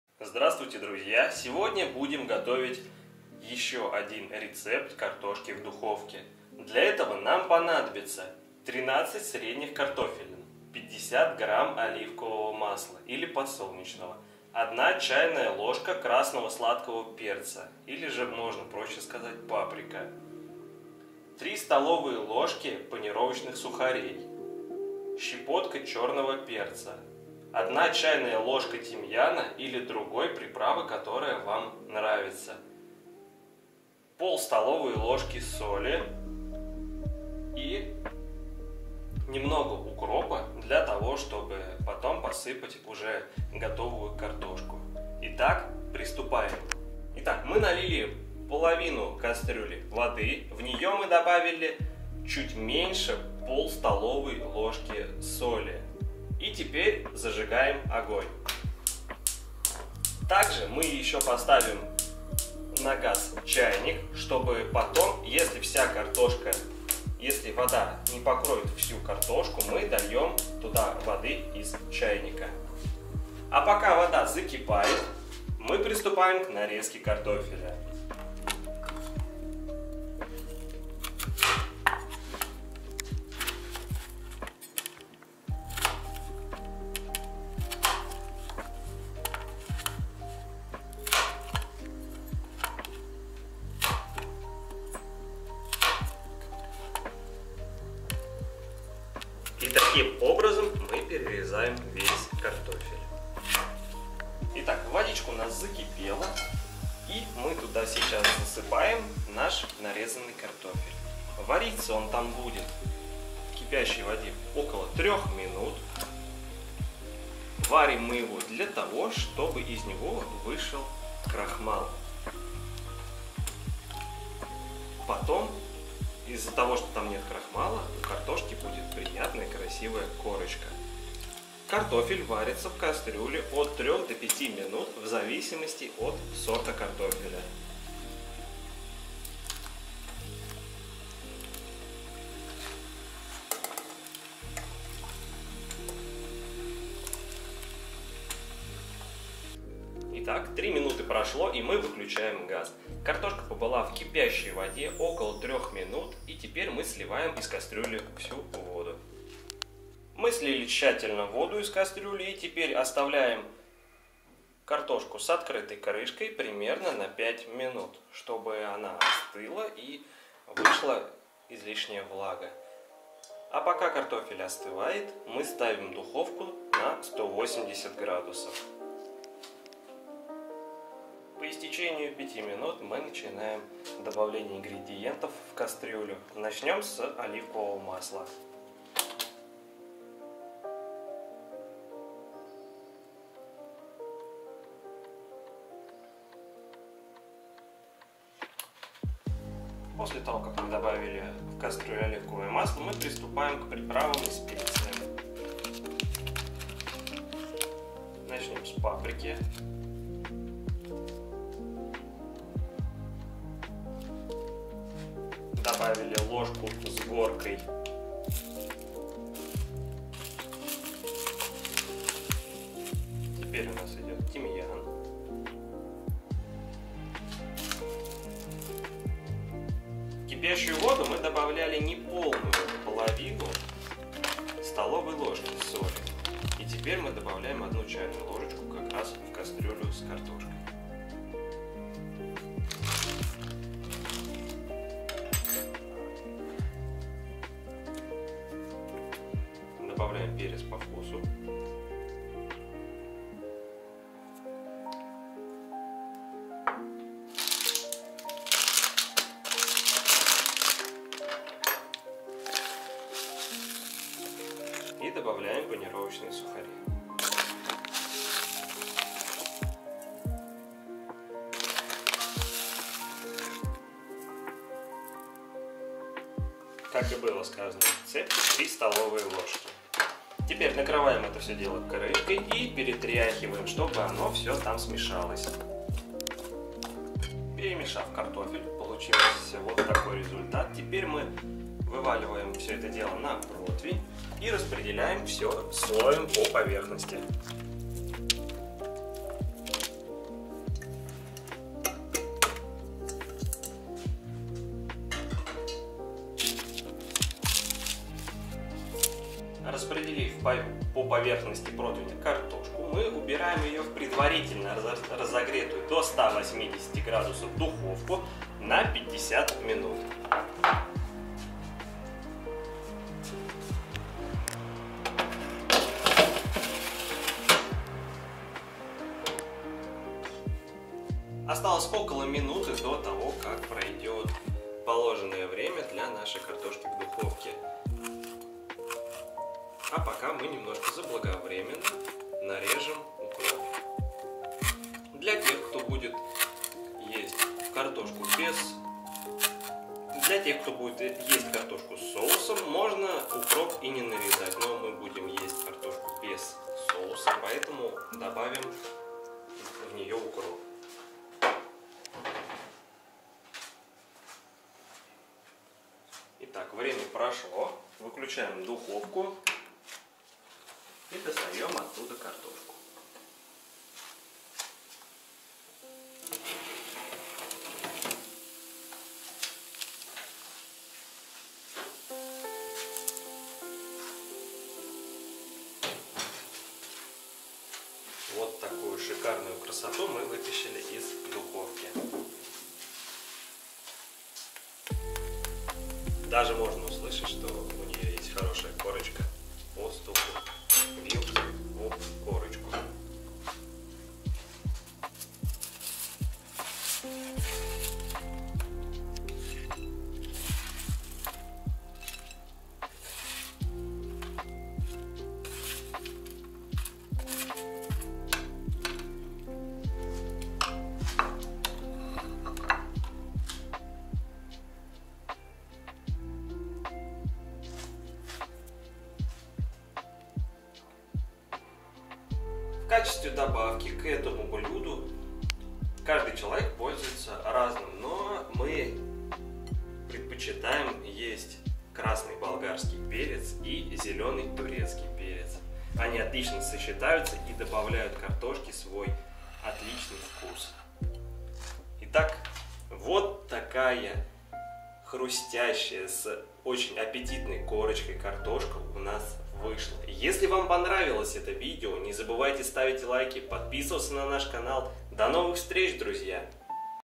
Картошка В Духовке - Запечем По Новому! КАРТОФЕЛЬ по деревенски
Сегодня приготовим один из самых интересные рецептов картошки!